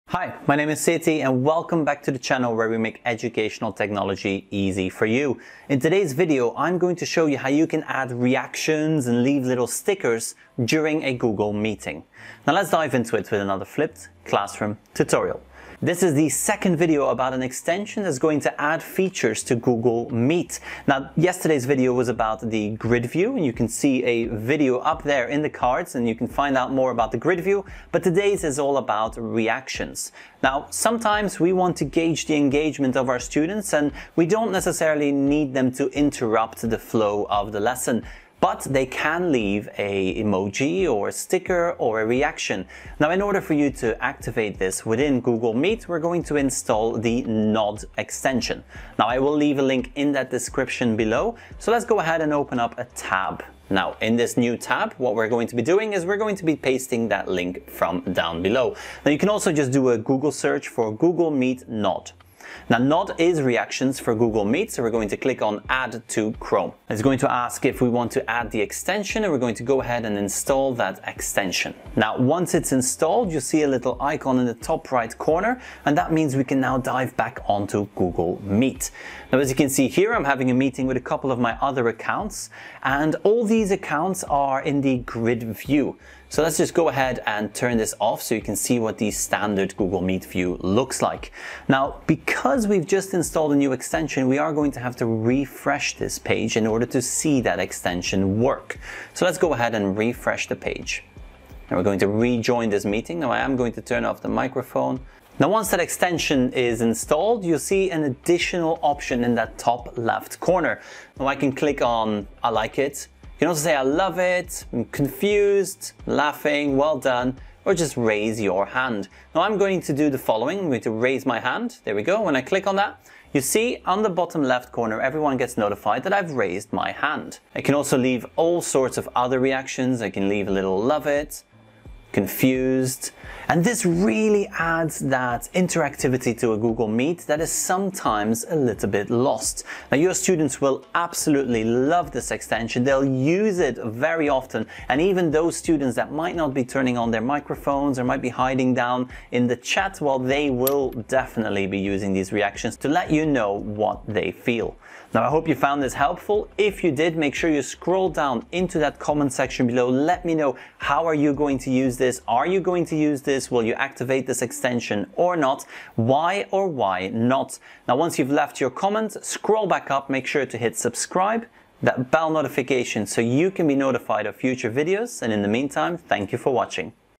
Reactions and stickers in Google Meet
Have your students leave reactions without interrupting a lesson. Give your students a chance to visually show how they are understanding the content or how they are feeling. I love this extension, which will add reactions and emojis to the Google Meet platform! A Great feature for students engagement and a lot of fun!

(These really help make our content better at No extra cost to you)